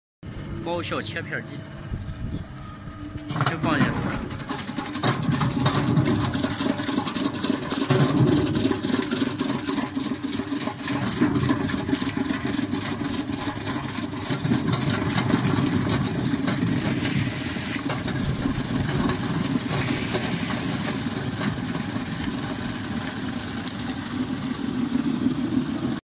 Cutting with water tank
Joyshine Potato Carrot Eggplant cutting machine/Selina Chen
1. High efficiency, simple appearance and structure, easy to operate
2. Fast cutting speed, cutting surface is smooth, without damage or crack
3. Cutting strips sizes adjustable from 7*7 to 12*12mm by changing the cutter knives
4. Discharging direction could be flat or bent down
5. Suitable for various kinds of root vegetable
6. SUS304 material